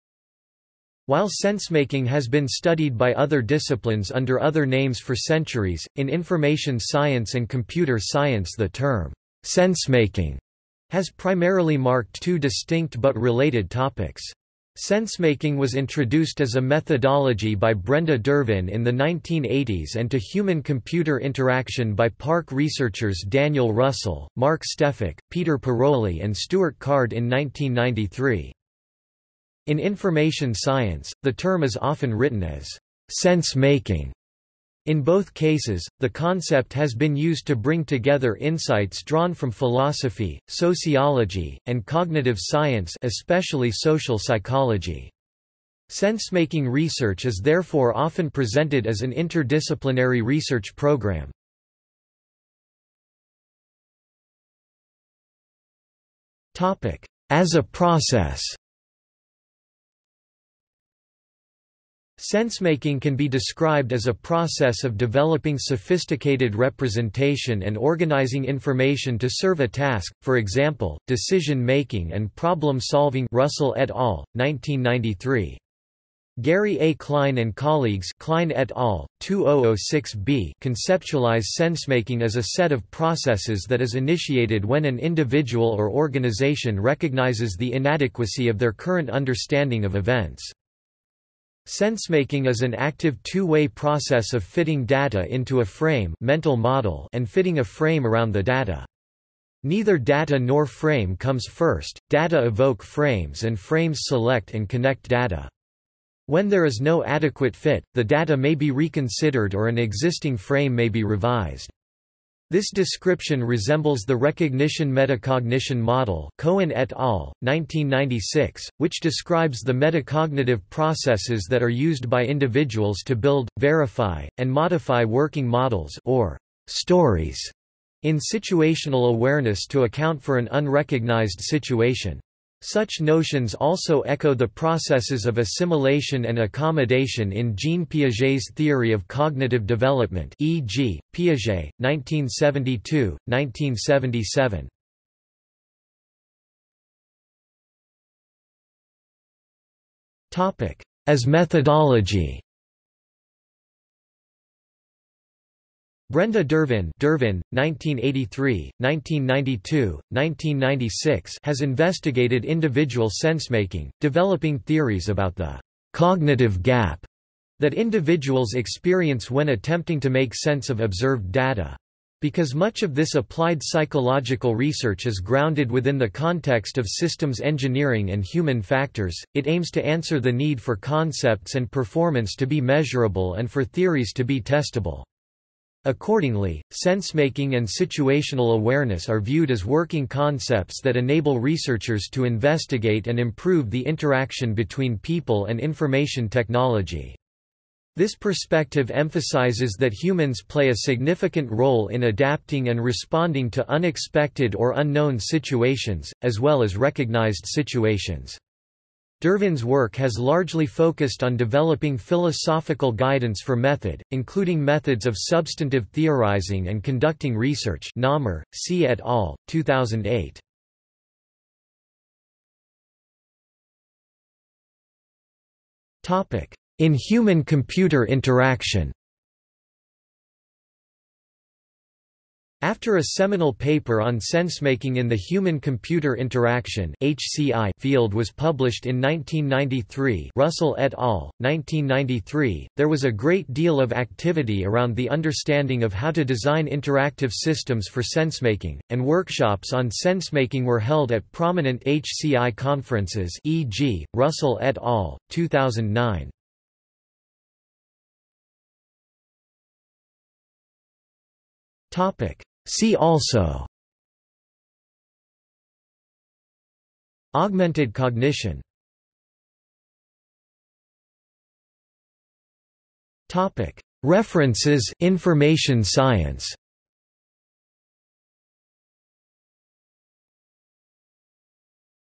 Sensemaking (information science) | Wikipedia audio article
00:00:52 1 As a process
00:02:26 2 As methodology
00:03:42 3 In human–computer interaction
00:04:18 4 See also
00:04:29 5 References (information science)

- Socrates

SUMMARY
While sensemaking has been studied by other disciplines under other names for centuries, in information science and computer science the term "sensemaking" has primarily marked two distinct but related topics. Sensemaking was introduced as a methodology by Brenda Dervin in the 1980s and to human–computer interaction by PARC researchers Daniel Russell, Mark Stefik, Peter Pirolli and Stuart Card in 1993.
In information science, the term is often written as "sense-making". In both cases, the concept has been used to bring together insights drawn from philosophy, sociology, and cognitive science (especially social psychology). Sensemaking research is therefore often presented as an interdisciplinary research programme.